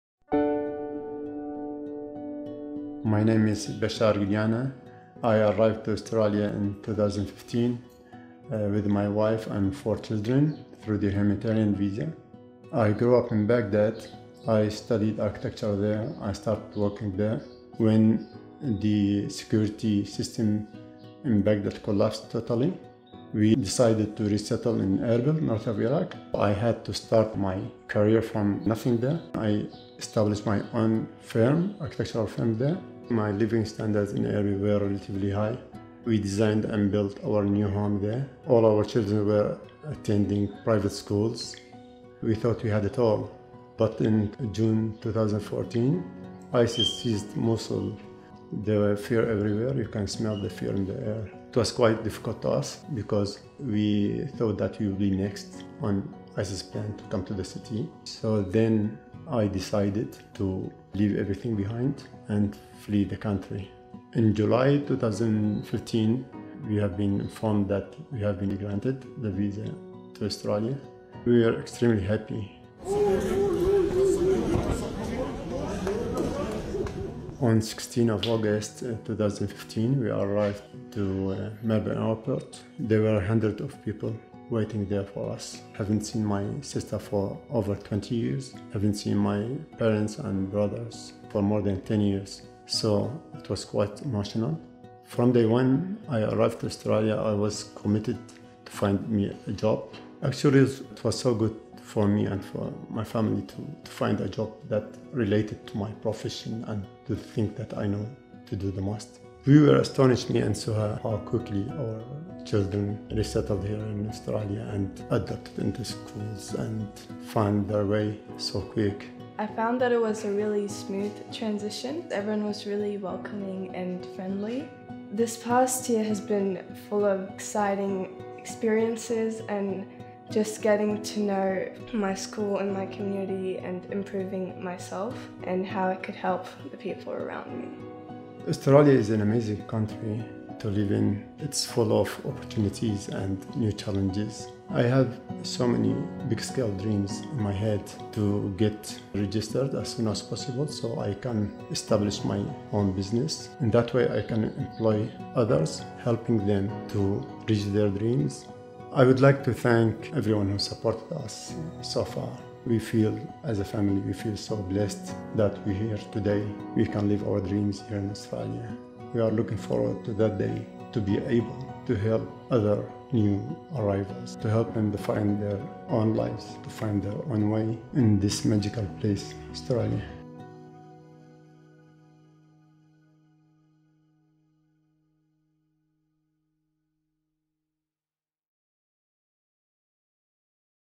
Bashar’s story—Refugee Week 2017
Bashar and his family were forced to flee Iraq in 2014 due to fear of being persecuted simply for their religious beliefs. He is now contributing back to Australia through his work as a draftsman.